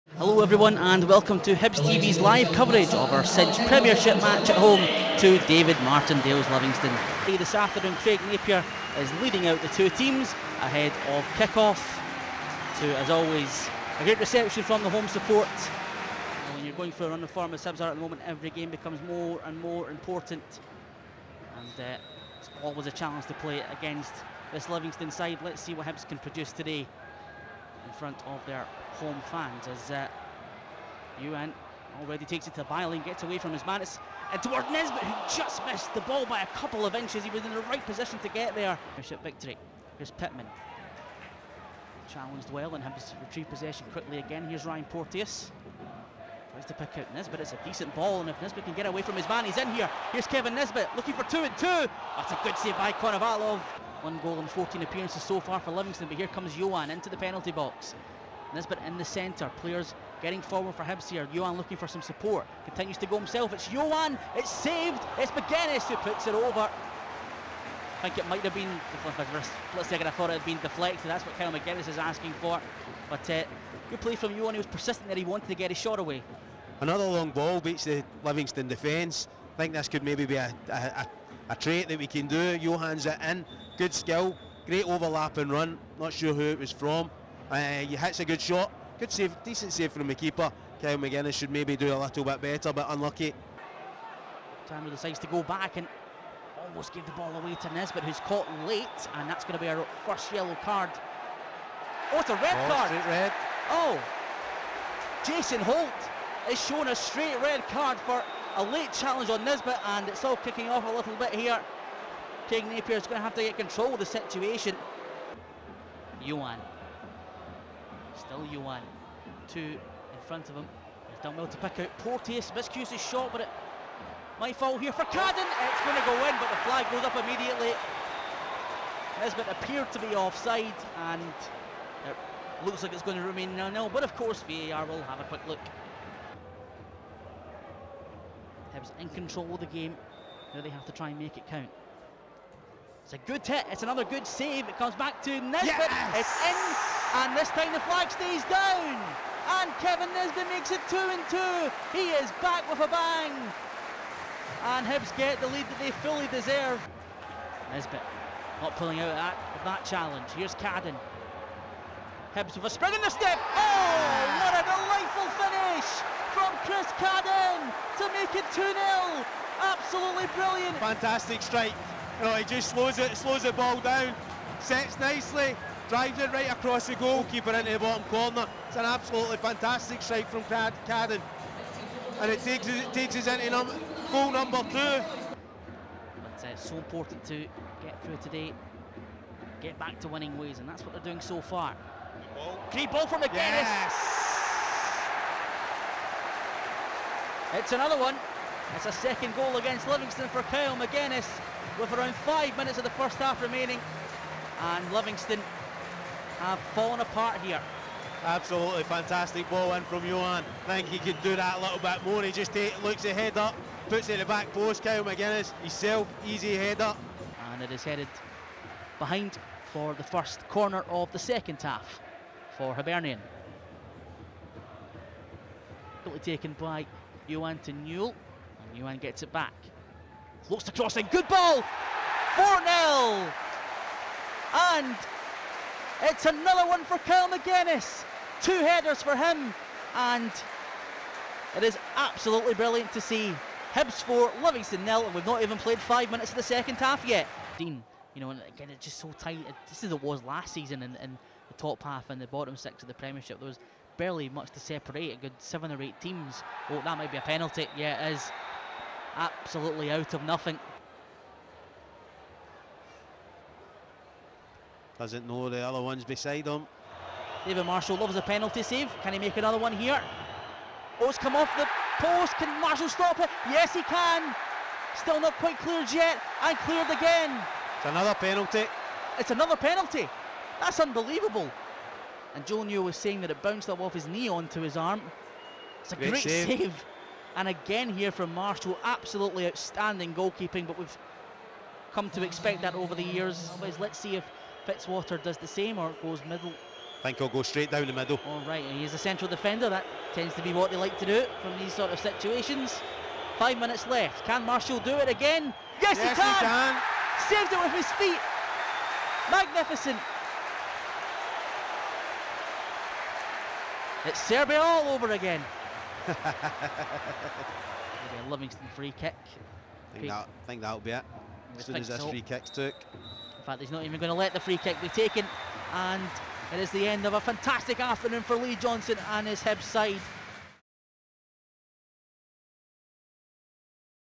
Highlights: Hibernian 4 Livingston 0 | cinch Premiership
Highlights from our 4-0 win over Livingston at Easter Road on Christmas Eve.